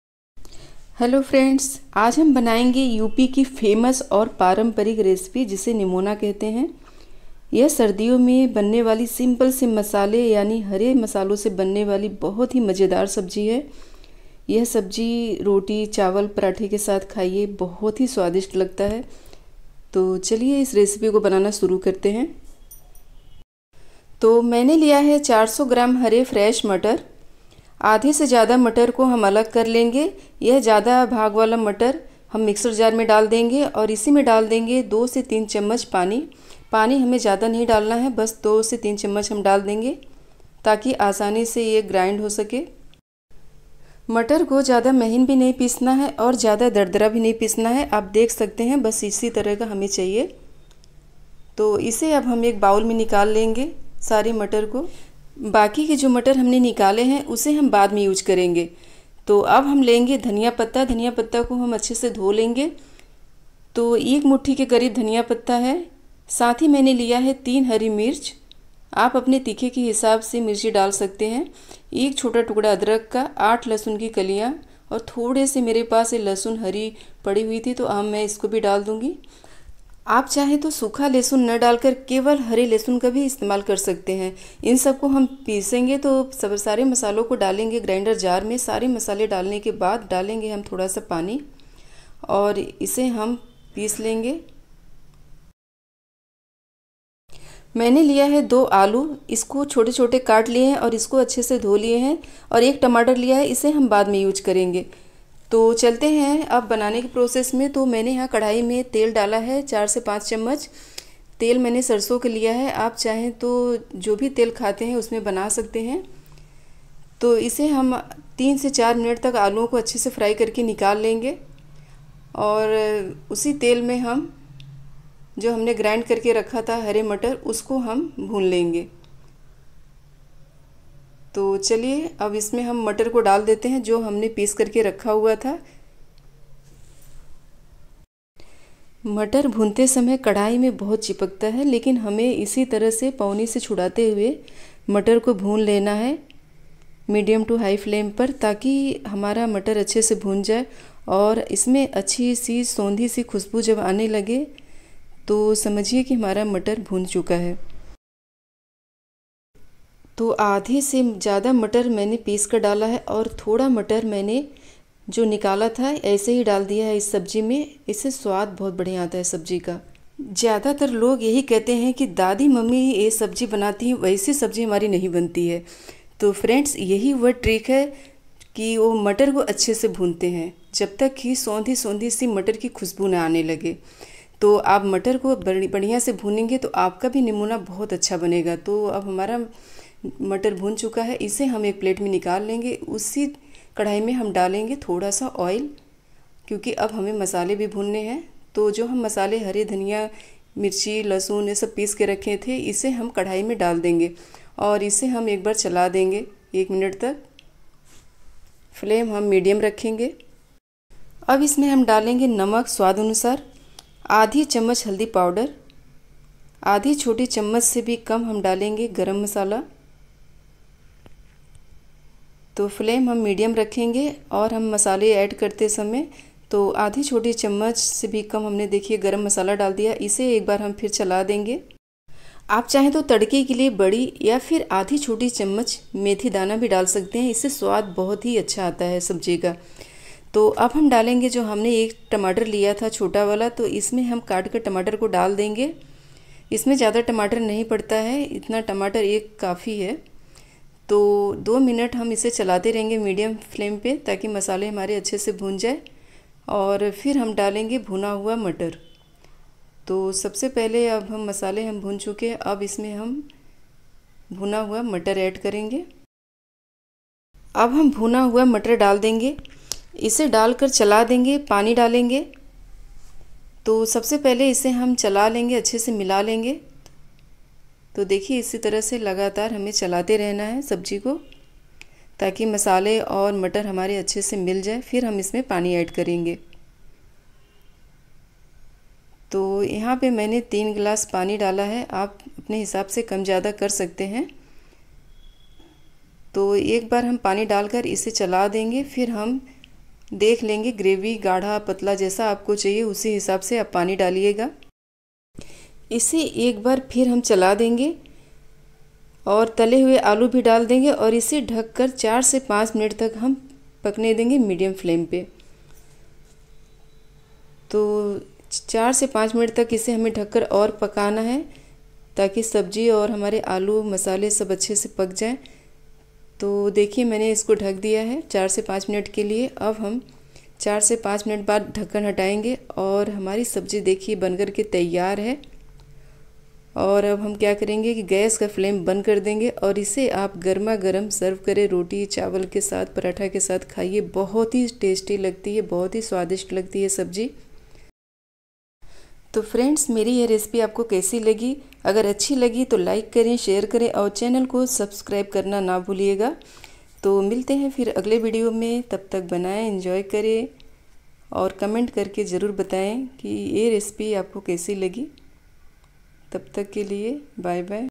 मटर आलू का निमोना बनाए खास ट्रिक के साथ | HOW TO MAKE NIMONA| यूपी की फेमस और पारंपरिक रेसिपी
nimona kaise banaye
easy recipefor up style nimona
viral recipe
famous food
foodie
traditional food